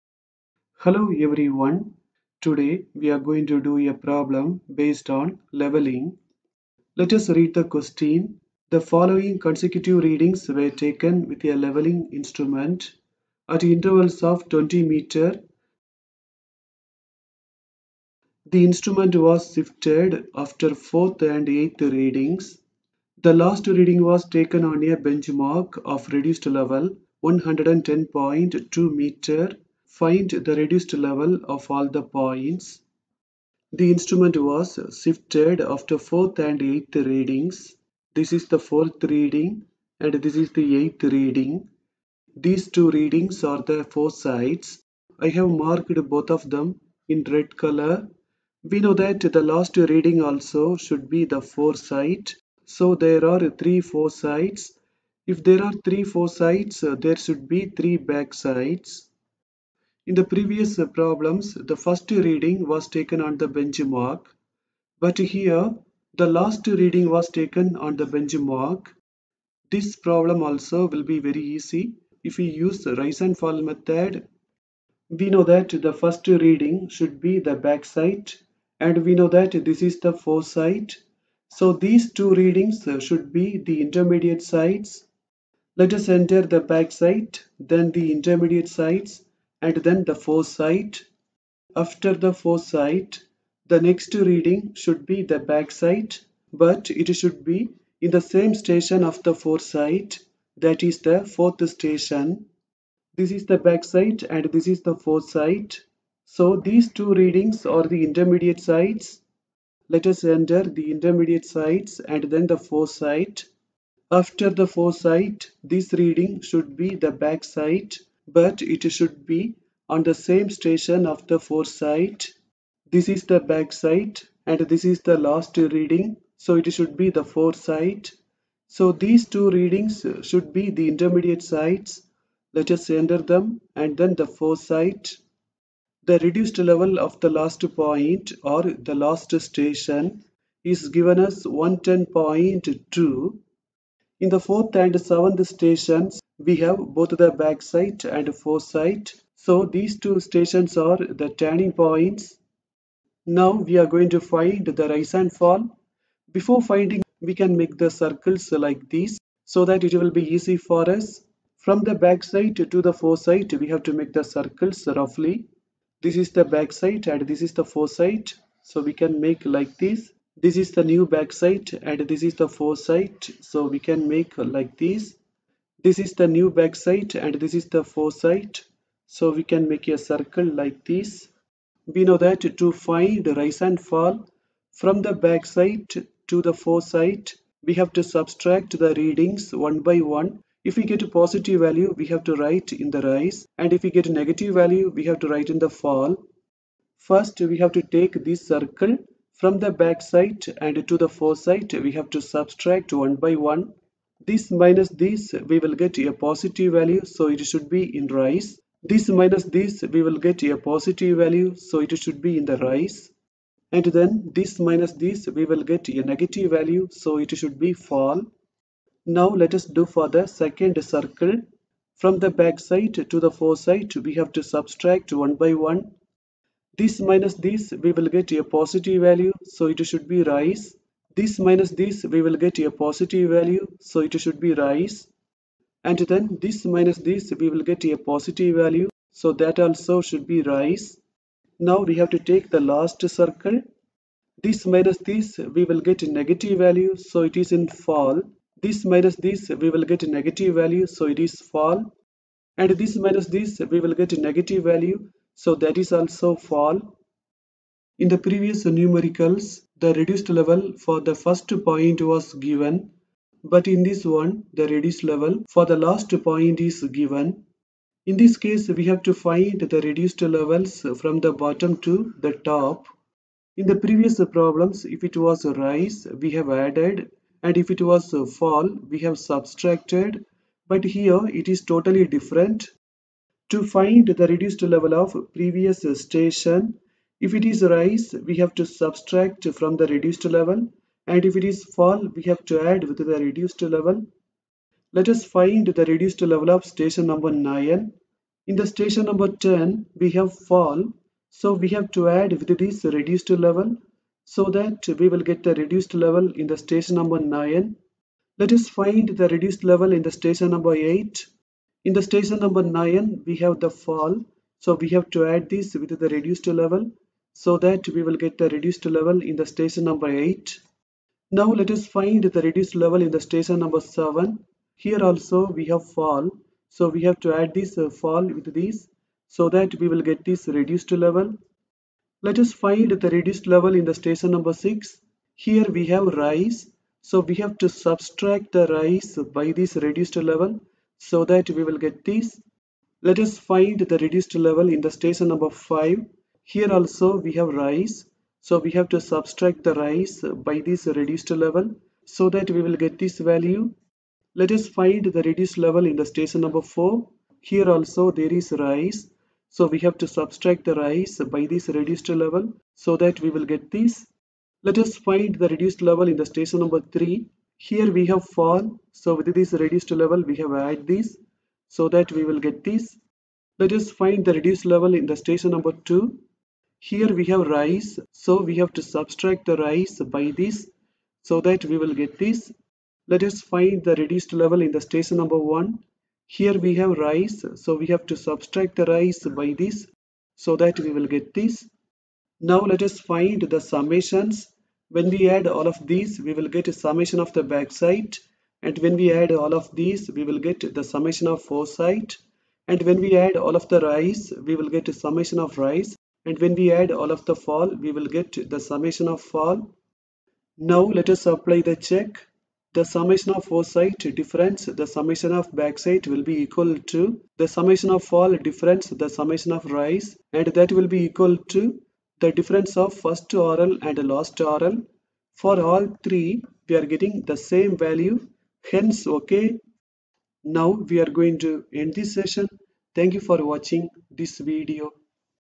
Levelling Problem No 5 - Benchmark Reduced Level given in the last station
The following consecutive readings were taken with a levelling instrument at intervals of 20m. The readings are 2.375, 1.730, 0.615, 3.450, 2.835,  2.070, 1.835, 0.985, 0.435, 1.630, 2.255 and 3.630 m. The instrument was shifted after fourth and eighth readings. The last reading was taken on a BM  of R.L 110.200 m. Find the  R.L of all the points.